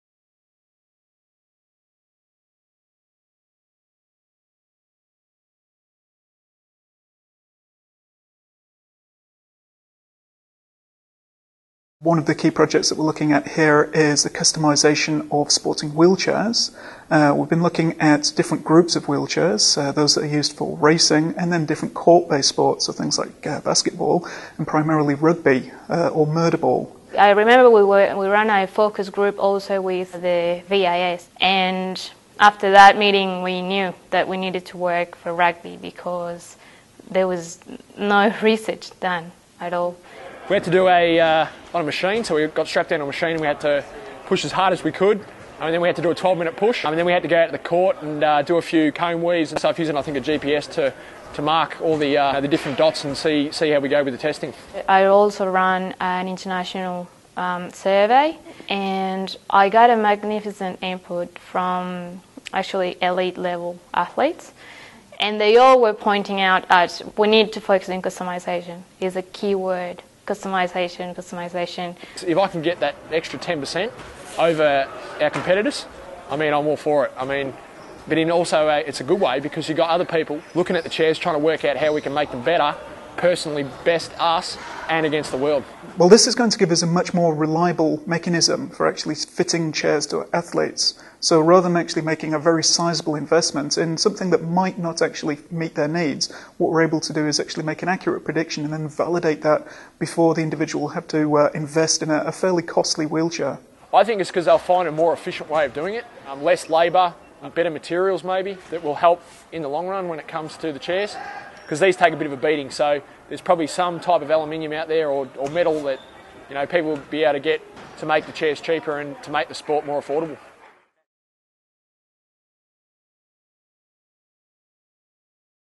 Wheelchair Rugby - RMIT University
RMIT University researchers are working with wheelchair rugby players to try and find a better way to customise chairs to improve comfort and performance.

With a sizeable investment in purchasing the chairs, the research aims to address the lack of a cost-efficient method of determining the best model chair for individual players prior to purchase.